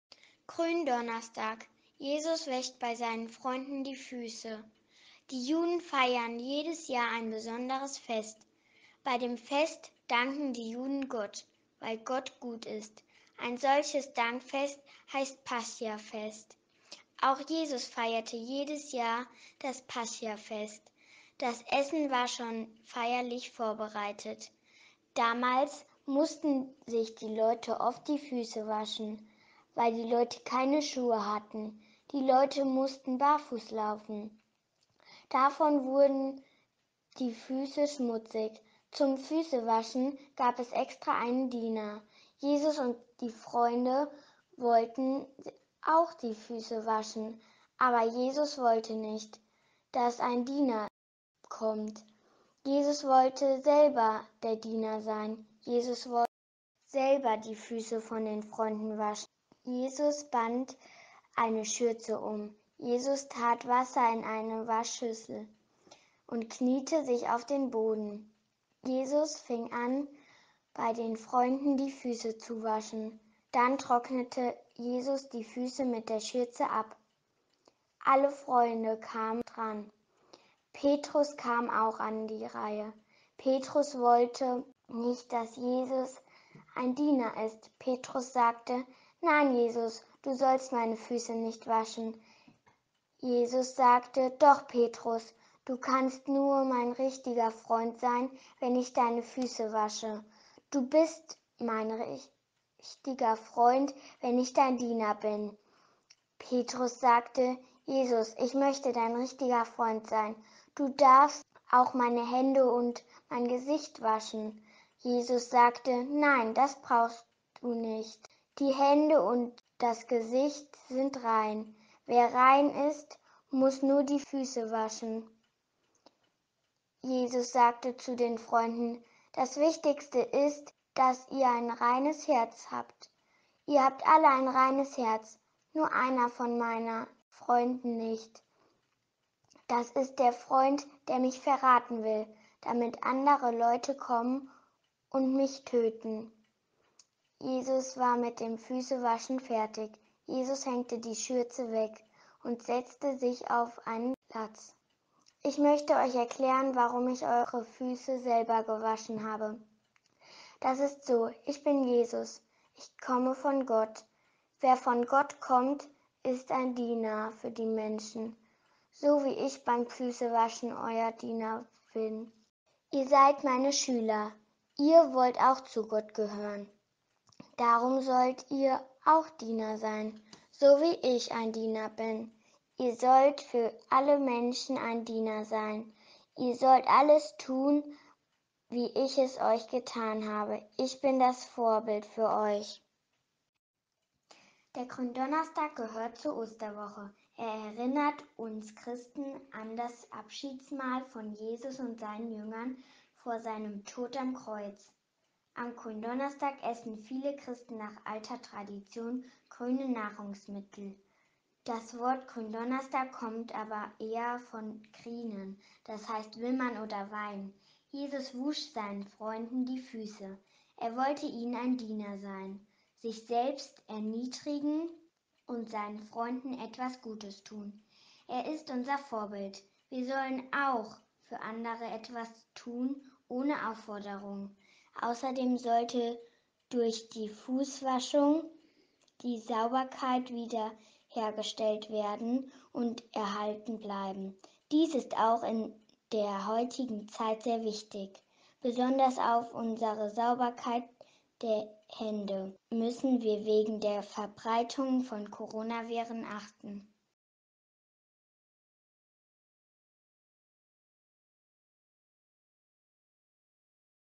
Glauben aus Kinderaugen - Gründonnerstag
Kommunionkind Johanna liest das Evangelium von Gründonnerstag vor und erklärt das Brauchtum